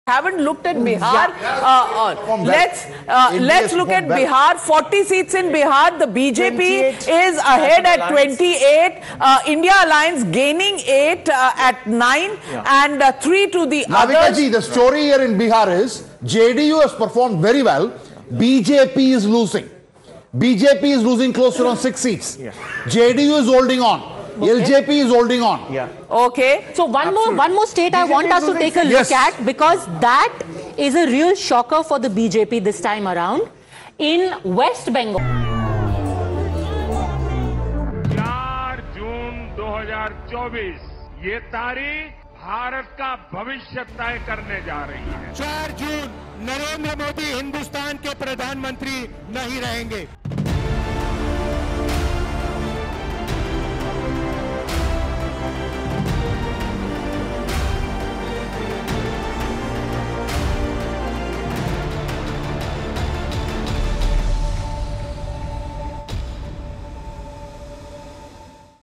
Lok Sabha Election Result 2024: JDU Takes Lead in Bihar, BJP Still Loosing | ET Now | Latest News
The nation waits with bated breath for the Lok Sabha election results, as Prime Minister Narendra Modi seeks to achieve a historic third consecutive term. The opposition coalition, INDIA, aspires to cause an upset, but the odds appear to favor the BJP-led National Democratic Alliance (NDA). The vote counting marks the conclusion of an extensive polling process that spanned over 80 days.